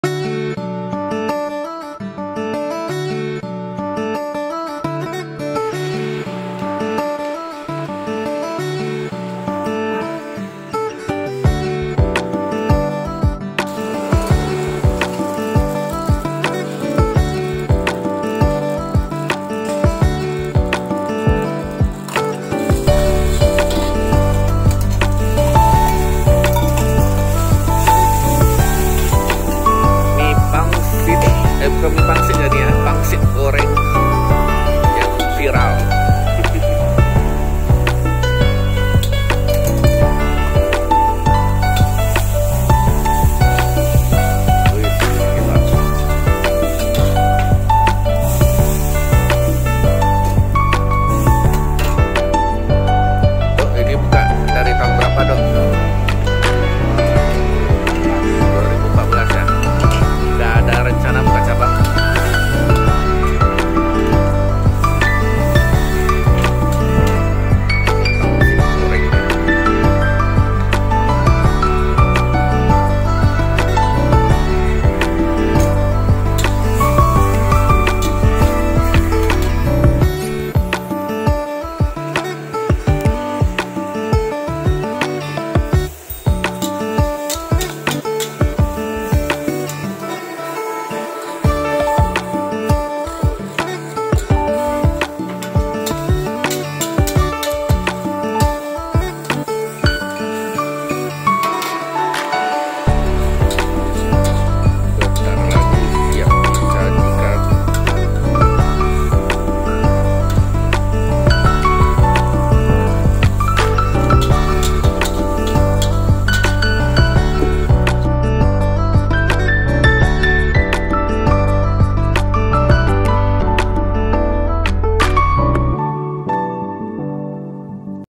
Kuliner Pekanbaru : Hrs dicoba Pangsit Goreng yg viral di Kedai Kopi MM Rasa enak bikin nagih...
KEDAI KOPI MM
Jl. Lokomotif No. 103B
Pekanbaru